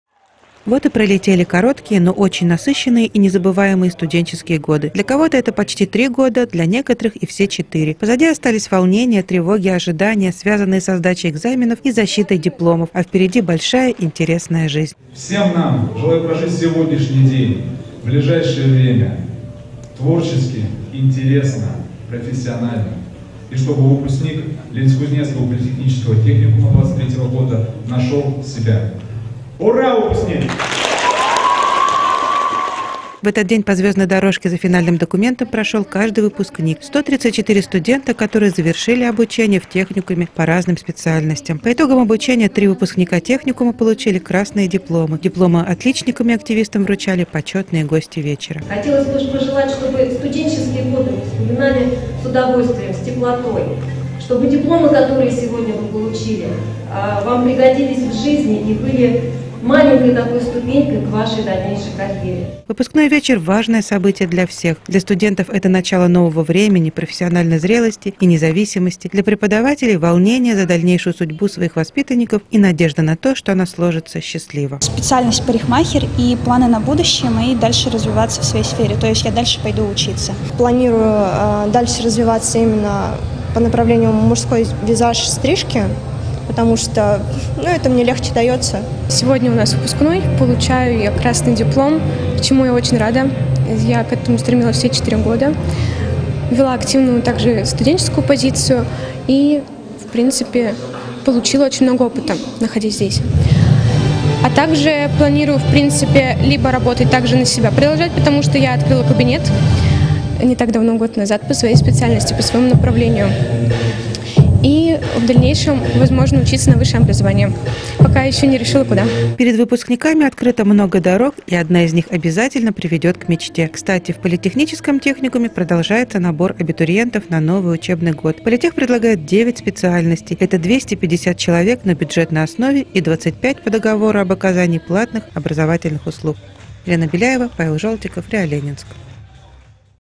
Выпускной в ЛКПТ
Выпускной в Ленинск-Кузнецком политехническом техникуме стал ярким завершением учебного года. В обновленном актовом зале учебного заведения прошло торжественное вручение дипломов. На празднике присутствовали почетные гости. Это заместитель главы Ленинск-Кузнецкого городского округа по социальным вопросам Евгения Мариненко и представитель министерства образования Кузбасса Анна Скворцова.